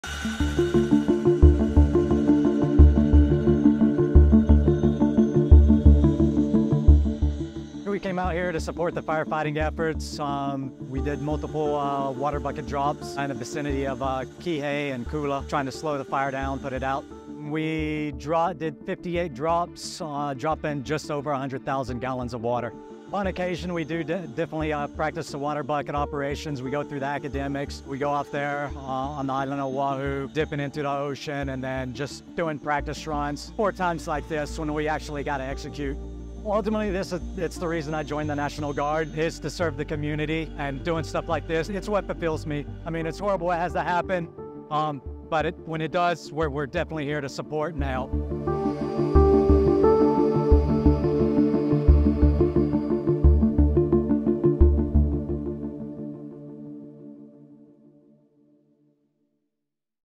National Guard aviators battle devastating wildfires in Hawaii
Two Hawaii Army National Guard CH-47 Chinook helicopters were deployed Aug. 9, 2023, to help battle wildfires that have devastated the island of Maui. The air crews performed 58 total bucket drops in 5 hours totaling more than 100,000 gallons dropped on the fires. (U.S. National Guard Video by Air Force Master Sgt. Andrew Jackson, edited by Master Sgt. Amber Monio)